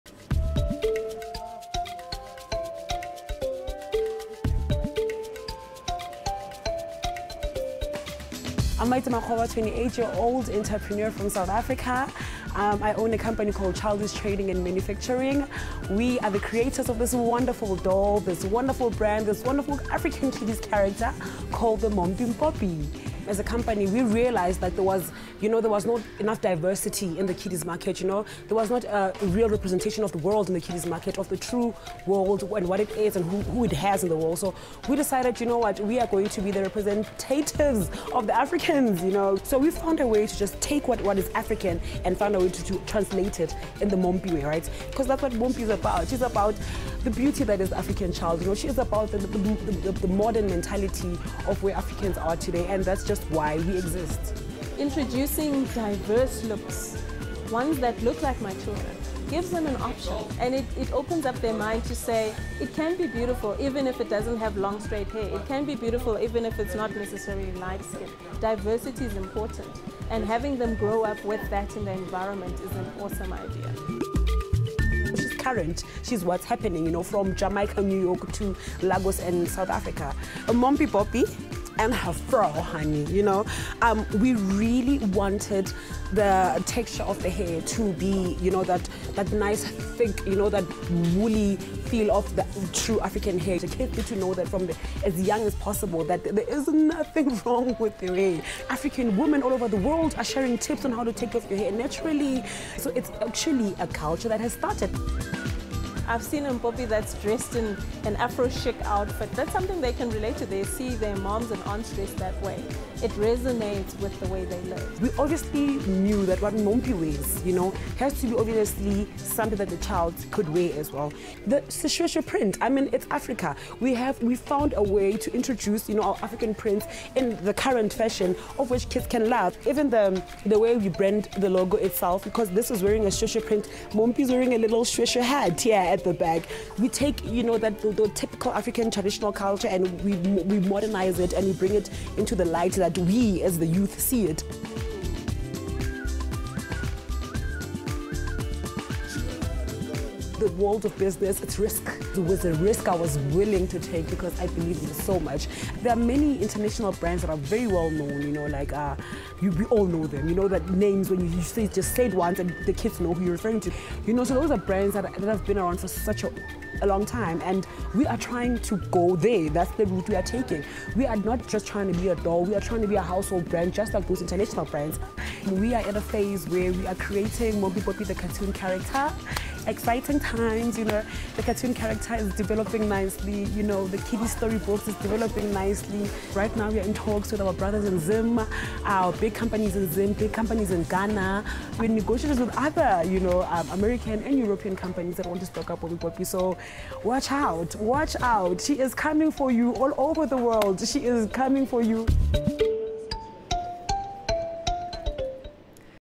South African doll entrepreneur
A new African doll is making waves for all the right reasons. Mommpy MPoppy has become hugely popular with young black girls in a market saturated with white dolls and its creator is planning on sending the dolls across Africa.